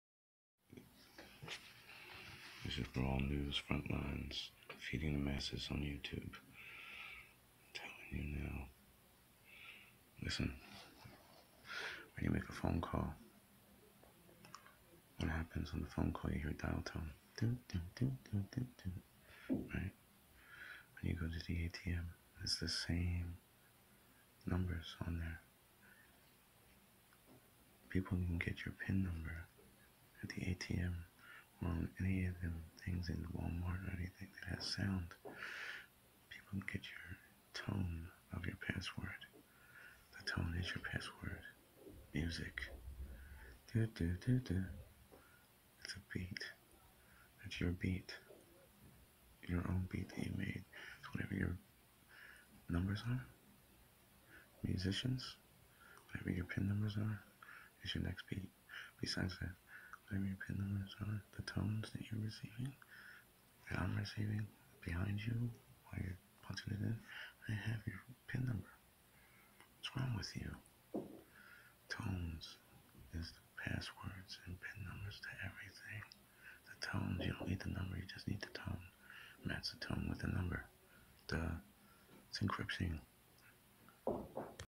ATM PIN Numbers Can be hacked by tones. and key pad numbers are all in Je
BANK PINS ARE NOT TONE DEAF.